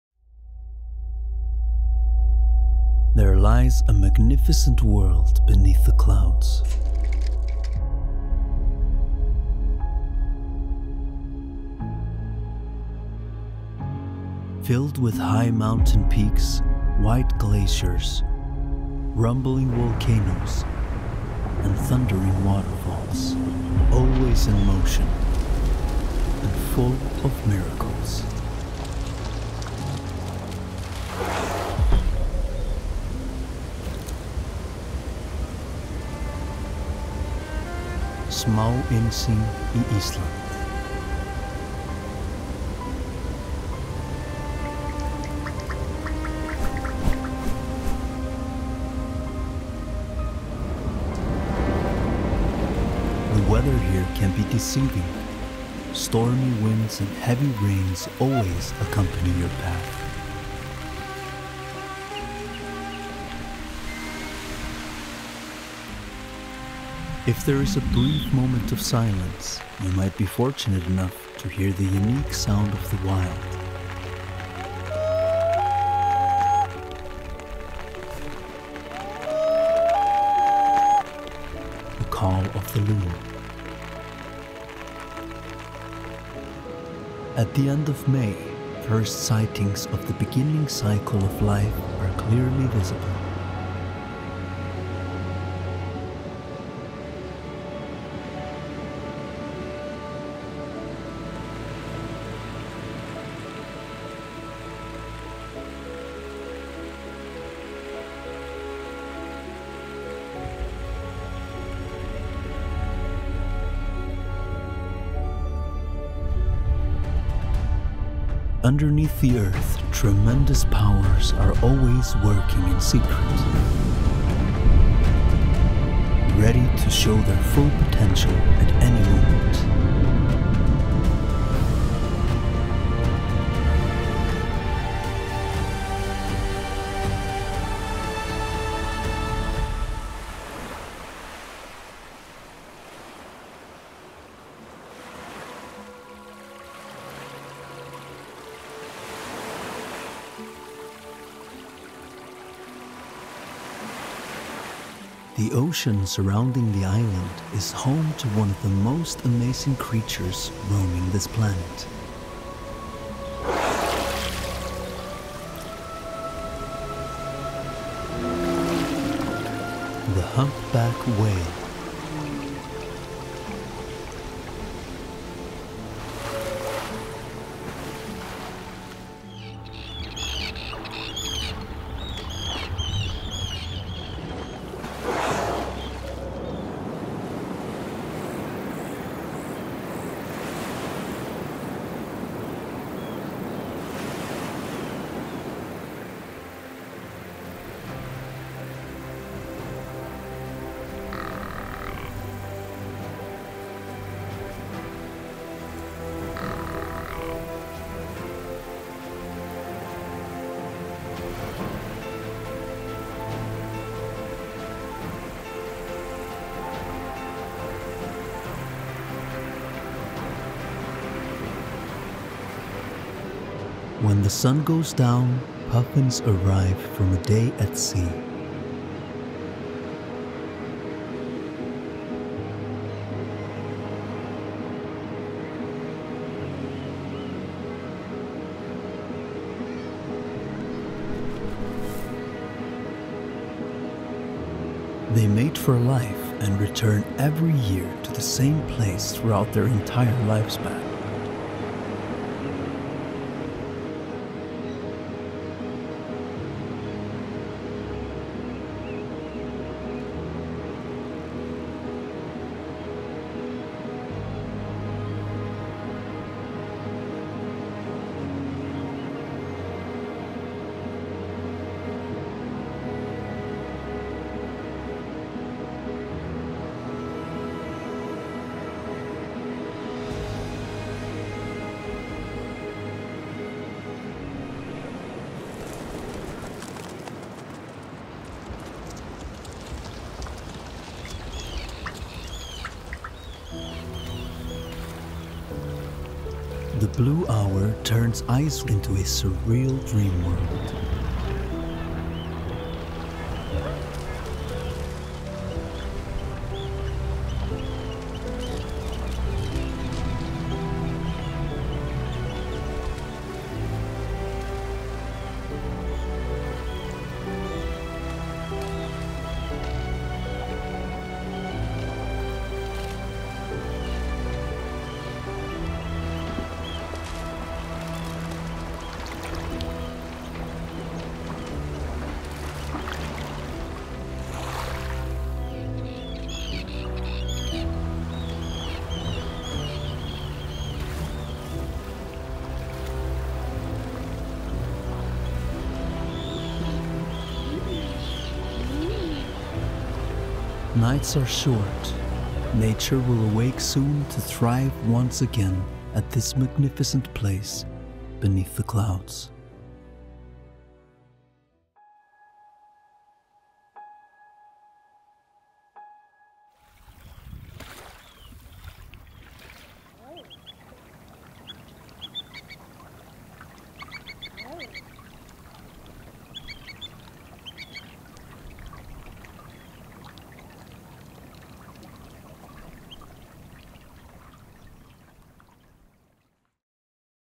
A GLIMPSE OF ICELAND | Cinematic Nature Short Film | SONY A7IV
A GLIMPSE OF ICELAND  - A cinematic nature short film.
Roaming the vast solitude of Iceland for only 12 days, I wanted to capture the feeling of getting just a glimpse of this pure and overwhelming world with an abundance of amazing creatures and stunning landscapes. Solo traveling gave me the opportunity to fully concentrate on filmmaking as much as possibly, resulting in this six and a half minutes long short film. Main goals of my cinematic journey were especially animals like the common loon, humpback whales and the adorable puffins, which all made it into the final film, but also the breathtaking sceneries of Iceland.
🎥 Filmed w/ Sony A7III | Sony A7IV | Sony 200600 | Tamron 28-75 | DJI Air2s
🎨 Edited in Premiere Pro, colors in DaVinci Resolve, music from Motion Array.
📍Iceland: Reykjavik, Vik, Husavik, Jökulsárlón, Snæfellsnes, Borgarfjörõur & more.
Thanks to Ivan for the amazing voice over! Wanted to have a native Icelander for this part.